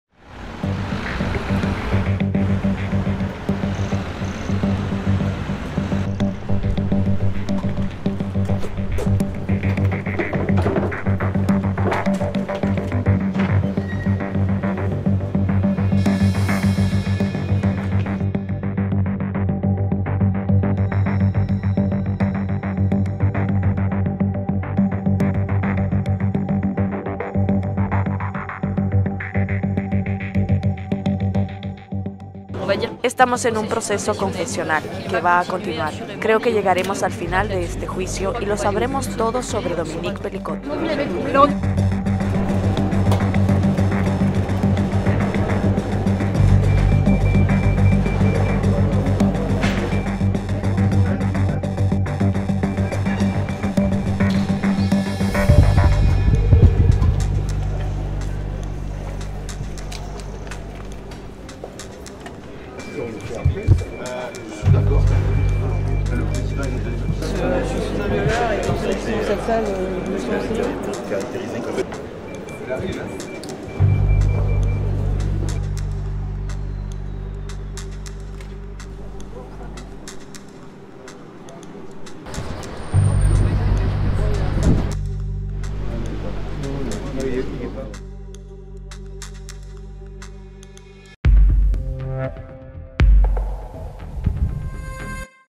El aberrante caso de Dominique Pelicot | El Colombiano
Dominique Pelicot, acusado de drogar a su mujer para que la violaran decenas de desconocidos, reconoció el martes ser "un violador", y afirmó que Gisèle Pelicot "no merecía" el calvario sufrido durante cerca de diez años.